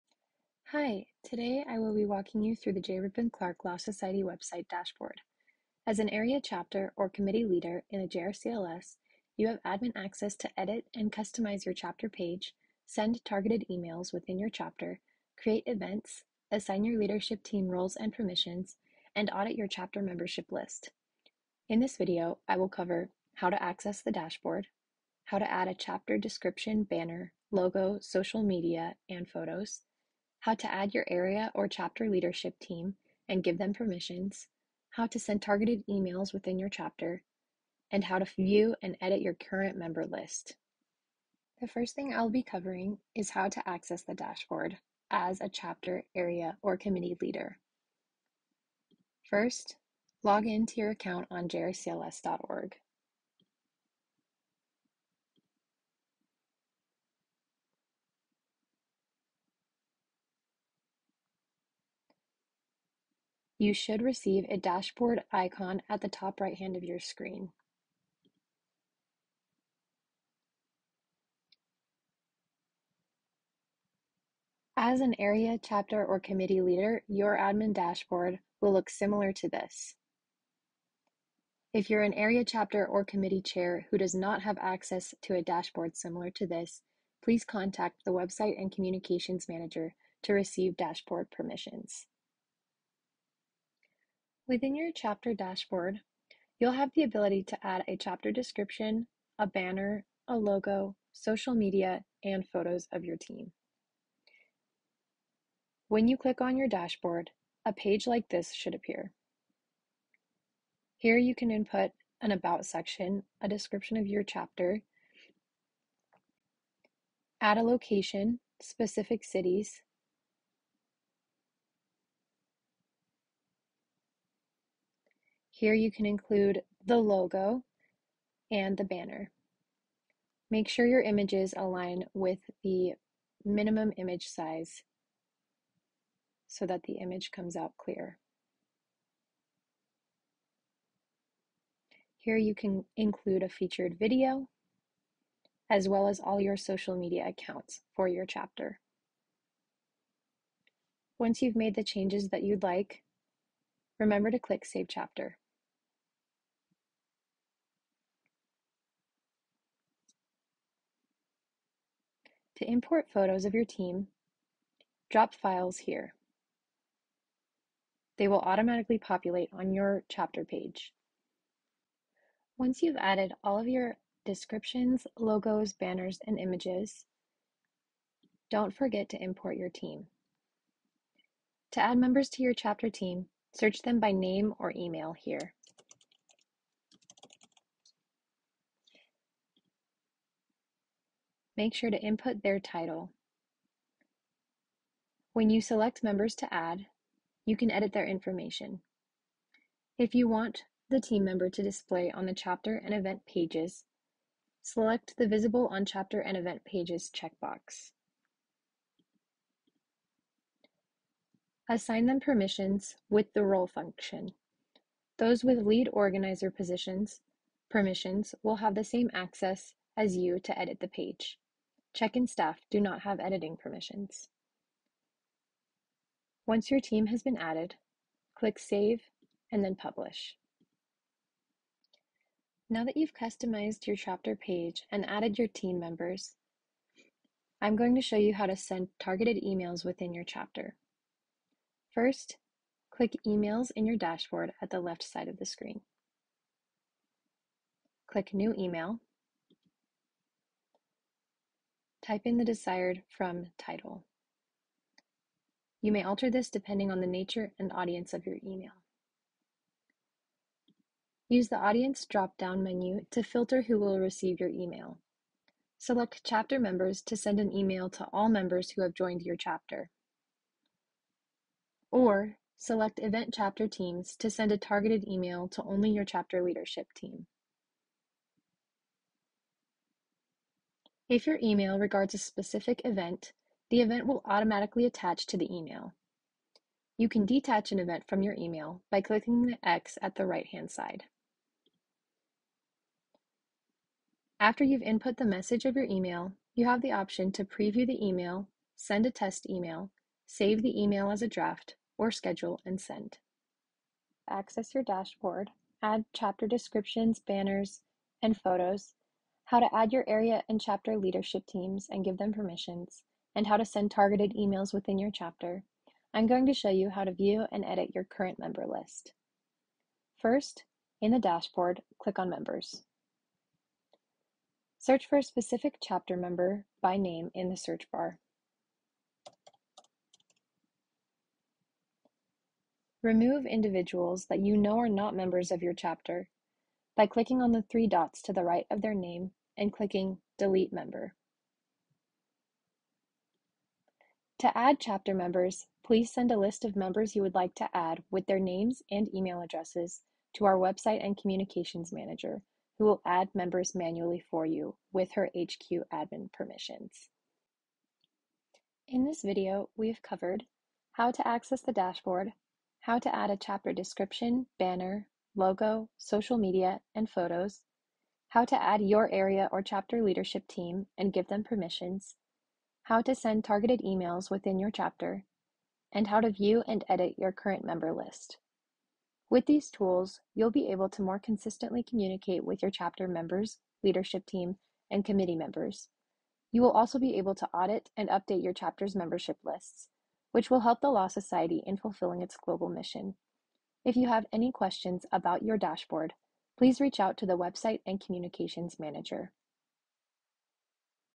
JRCLS | Leadership Dashboard Training Video
0:00-0:41 - Intro
0:42-1:33 - How to access the dashboard
1:34-2:51 - How to add a chapter description, banner, photos
2:52-3:51 - How to add team members
3:52-5:04 - How to send targeted emails
5:05-5:57 - How to view and audit your member list
5:58-6:41 - Conclusion